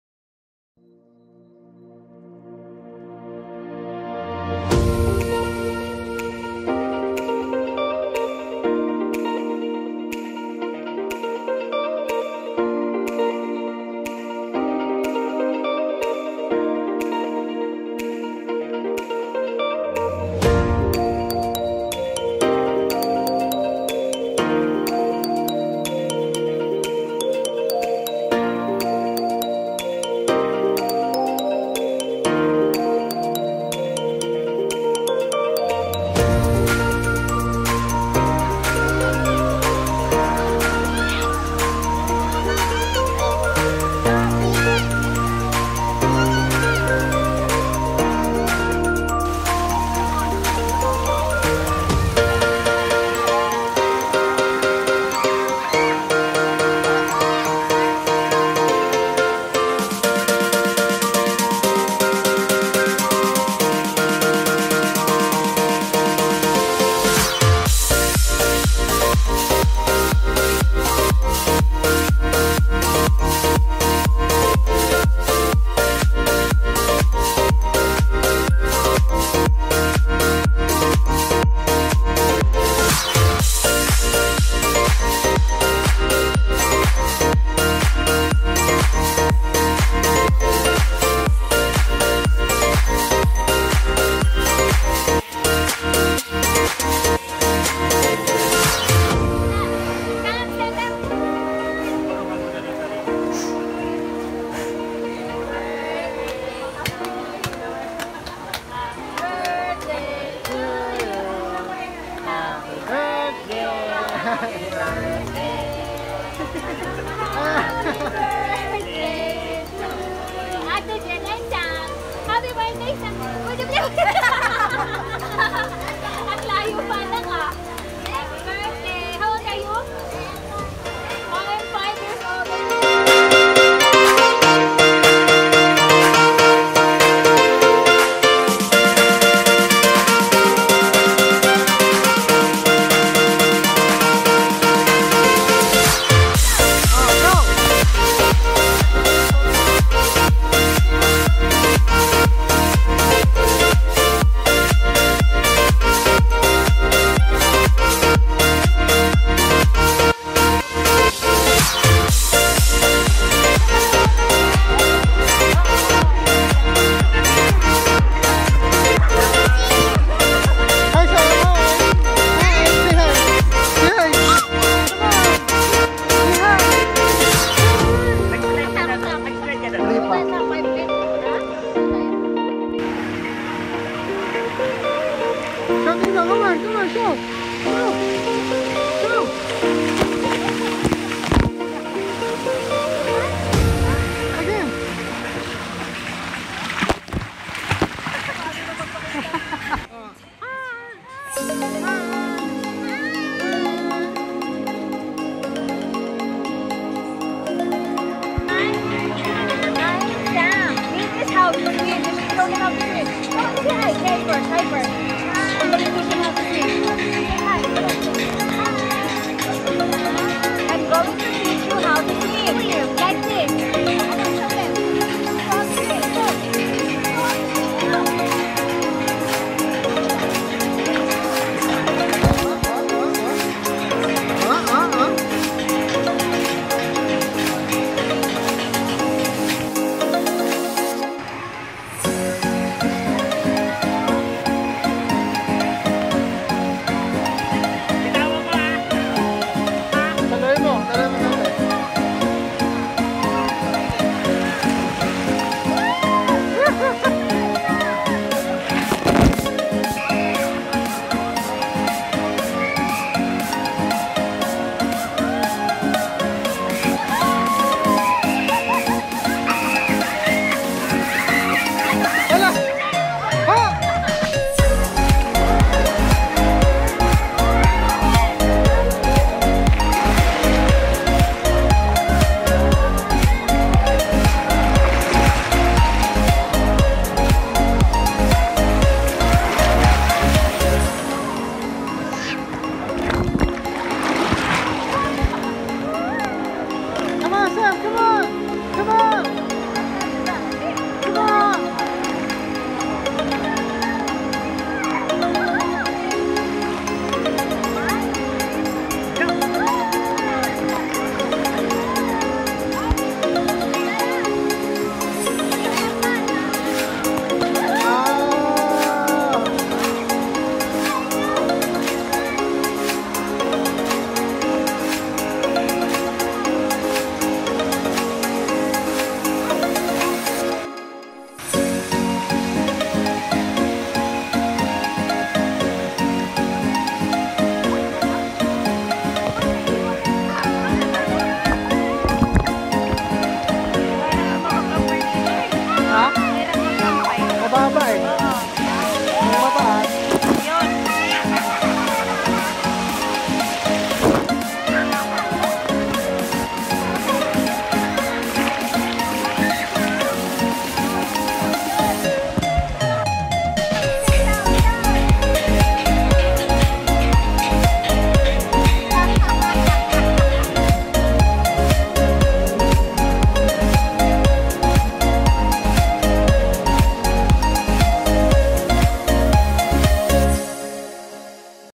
Sam & Jed's Birthday Celebration
Sam & Jed's Birthday Celebration at Watercamp Resort in Kawit Cavite together with the Bustillo / Poyos fam